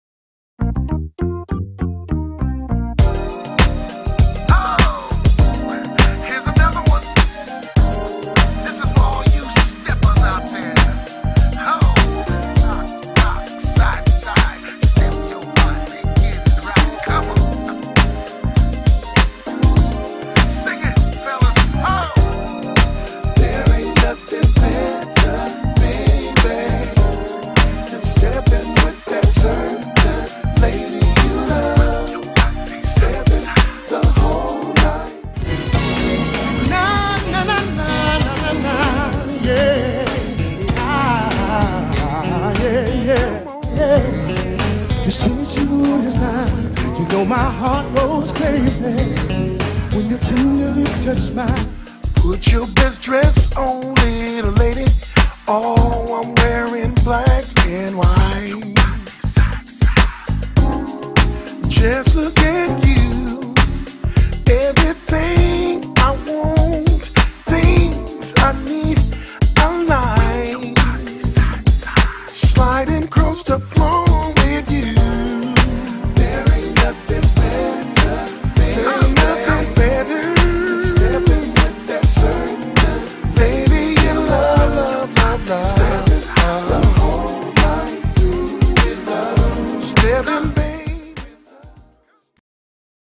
Always Remembered 2014 Robert F Brookins Earth Wind and Fire online video cutter com
"Always Remembered" 2014 Robert F. Brookins. June 14th at The Lion's Gate Hotel watt ave McClellan AFB.